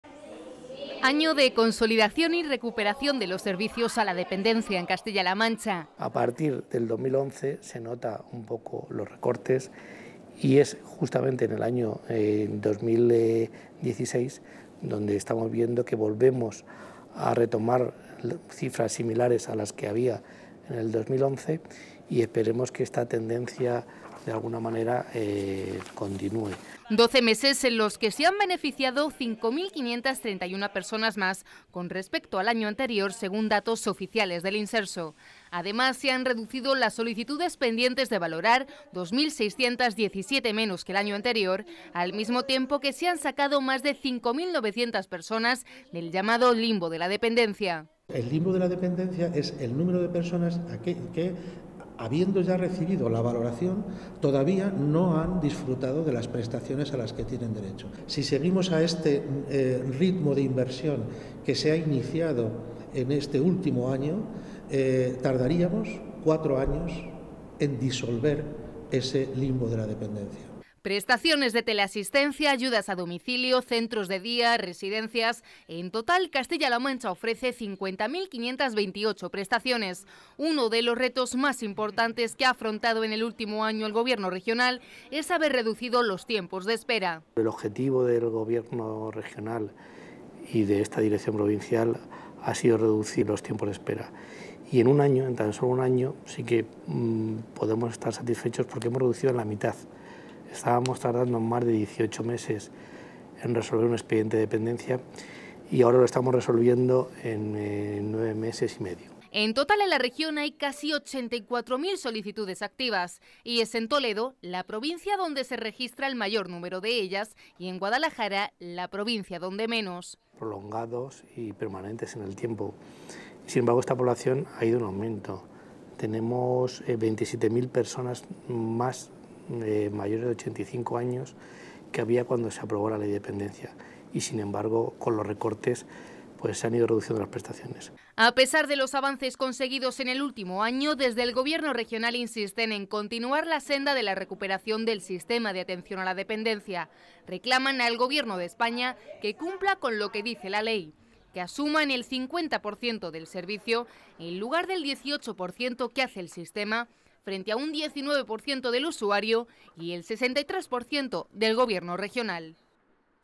5.000 PERSONAS MÁS QUE EL AÑO PASADO DISPONEN DE SERVICIO A LA DEPENDENCIA EN CASTILLA-LA MANCHA
Mejorar la calidad de vida de las personas enfermas, facilitarles una igualdad de oportunidades o ayudarles en la realización de actividades diarias, son algunos de los objetivos que llevan a cabo los servicios de atención a la dependencia.  Castilla-La Mancha ofrece más de 50.500 prestaciones que han beneficiado en el último año a más de 40.200 personas.